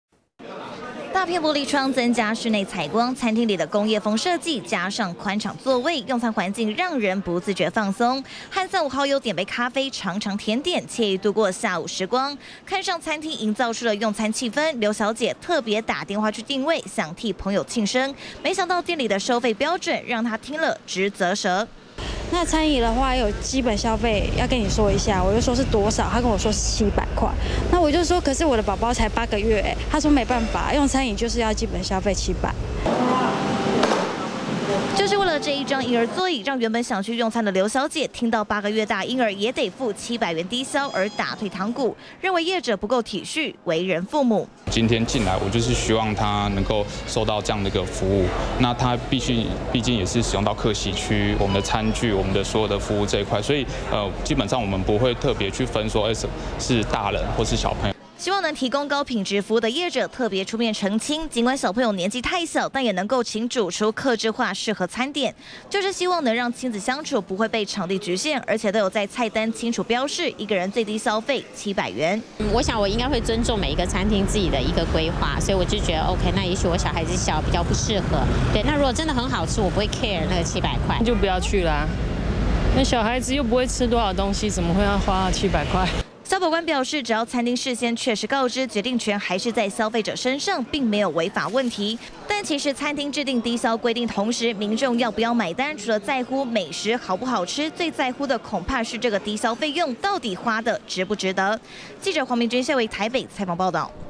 20150714中天新聞 餐廳制定最低消費 八個月嬰也要700
外出用餐我們都會看到業者特別標註，每位民眾的最低消費是多少錢，但有消費者向東區一間餐廳訂位時，被告知當天若帶小朋友去，也必須支付相同的低消費用700元，不過她的小朋友是不滿八個月大的嬰兒，讓她覺得實在是強人所難。業者對此強調，為了提供高品質服務，因此只要使用到座位都得一視同仁，對民眾帶來的不便也深感歉意。